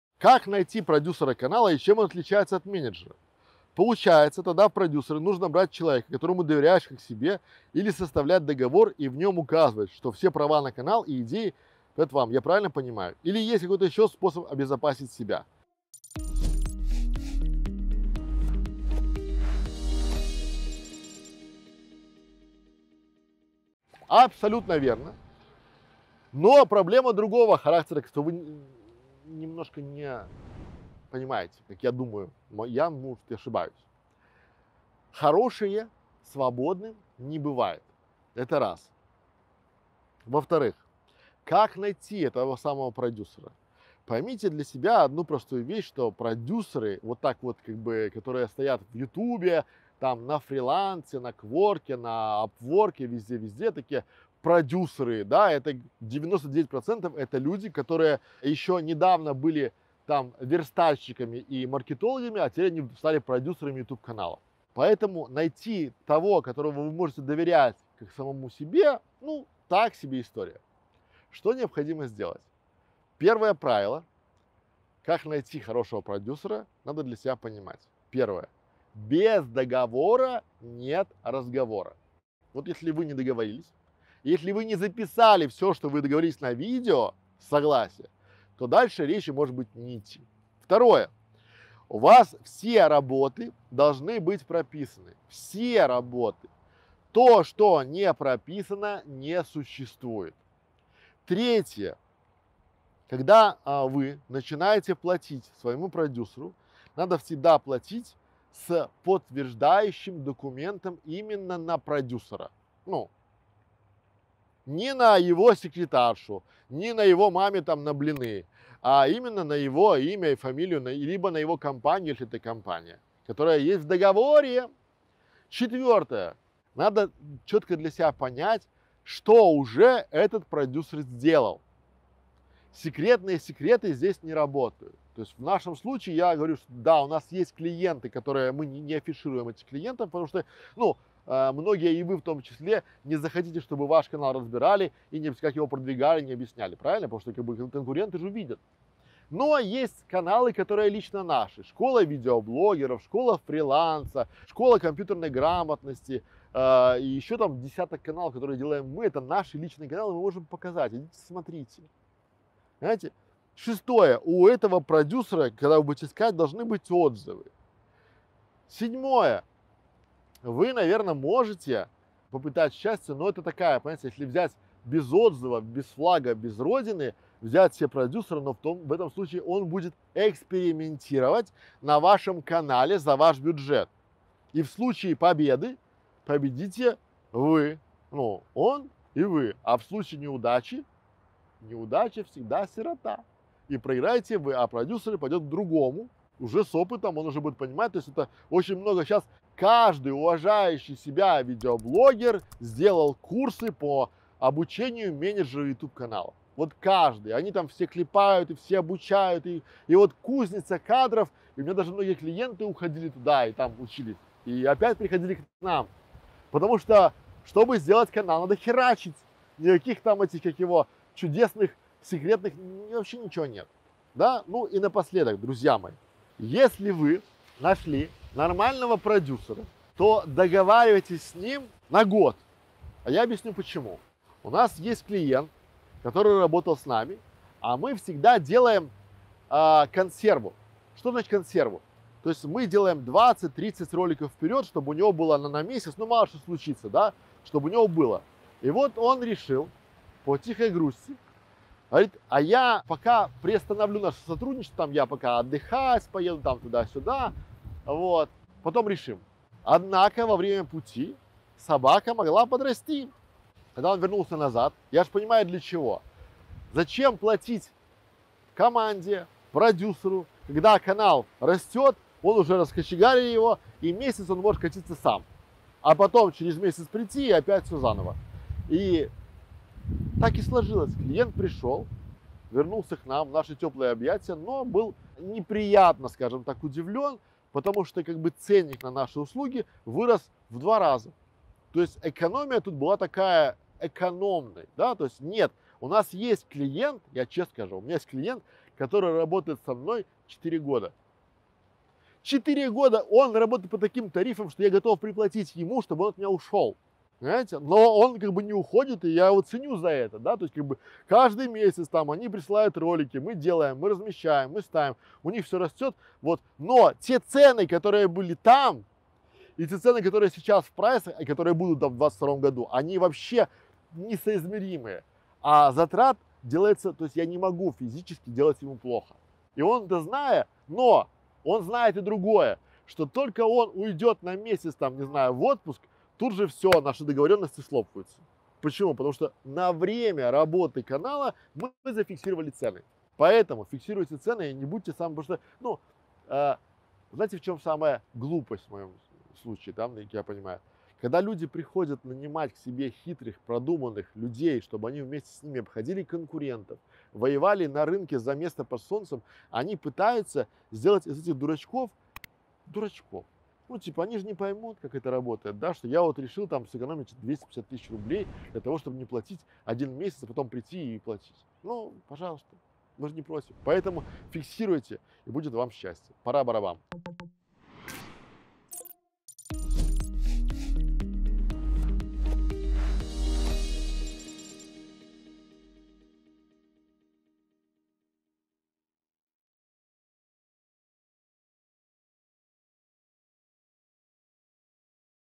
Как найти продюсера YouTube канала и чем он отличается от менеджера?
Поиск достойного персонала в команду — это всегда головная боль, хотя нет, головная боль это для тех, кто не знает где и как искать. Если вы будете знать основные моменты, на которые нужно обращать наибольшее внимание при подборе работников, то поиск станет в разы легче.

В данном видео Александр Некрашевич (продюсер YouTube канала) отвечает на вопрос одного из зрителей. Итак, вопрос следующий: "Как найти продюсера канала и чем он отличается от менеджера? Или есть ещё какой-то способ обезопасить себя? Друзья, договоры — это очень важная часть работы, они нужны всегда. "Без договора, нет разговора". Договор должен включать в себя несколько обязательных пунктов. А чтобы узнать каких, предлагаю вам досмотреть данное видео до конца. Узнайте это сейчас, чтоб потом не попасть в неприятную ситуацию.